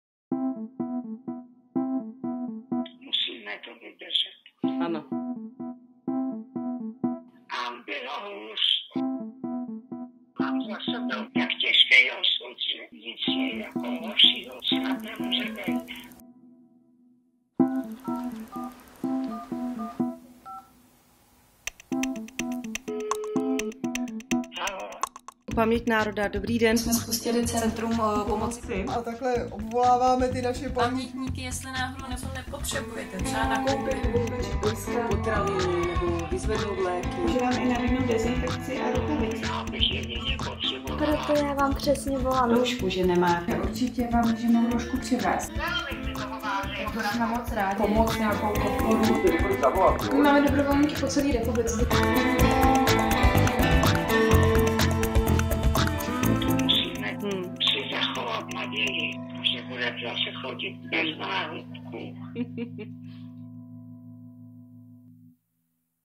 Centrum pomoci Paměti národa — Jak vypadá den některých z nás
Snažíme se pomáhat. Už měsíc funguje Centrum pomoci Paměti národa!

Lidé, kterým nabízíme pomoc, jsou v naprosté většině v psychické pohodě. Pokud se bojí, pak ne o sebe, ale o demokracii a svobodu. Ze zkušenosti ví, že strach pomáhá silám temna...
"Musíme to vydržet...bylo hůř...musíme si zachovat naději," řekla nám dvaadevadesátiletá paní Hyndráková, přeživší holokaustu a jedna z pamětnic, kterým jsme telefonovali a nabízeli pomoc. Nosíme nákupy, předáváme ochranné pomůcky, vyzvedáváme léky, opakovaně voláme. S nabídkou pomoci jsme oslovili pět tisíc seniorů. Pomáháme po celé zemi. Připojte se k nám i vy! Děkujeme 🙏
Velké díky všem, kteří činnost Centra pomoci podpořili, ať už svou prací na telefonu😉, svou energií, tím, že se přihlásili jako dobrovolníci nebo finančním či věcným darem.❤️ Neustáváme, budeme v aktivitě pokračovat stále dál.
📍 5 000 pamětníků jsme napřímo oslovili s nabídkou pomoci (dopisy, maily, telefony)
📍 fungujeme 24 hodin denně, 7 dní v týdnu po celé ČR, ve všech krajích
📍 476 dobrovolníků se zaregistrovalo po celé ČR
📍s 1758 pamětníky jsme mluvili telefonicky
📍 170 pamětníkům jsme pomohli nebo pravidelně pomáháme (dovoz ochranných pomůcek, nákup…)
📍 na nonstop telefonní lince pro pamětníky se střídá 7 operátorů na lince pro dobrovolníky další 2 operátoři
📍 pomoc jsme předali i dál: ochranné pomůcky (roušky, desinfekci, rukavice) jsme věnovali do Domu seniorů Hvězda, Domovu sociální péče Hagibor, Domov Sue Ryder, Městským sociálním a zdravotním službám Kolín
📍 o organizaci pomoci se stará 10 regionálních koordinátorů, většinou z kruhu našich přátel na pobočkách Paměti národa v Plzeňském kraji, na střední Moravě, ve východních Čechách, v Jihomoravském a Jihočeském kraji.
Chcete také podpořit Centrum pomoci Paměti národa, nebo se dozvědět více? Děkujeme.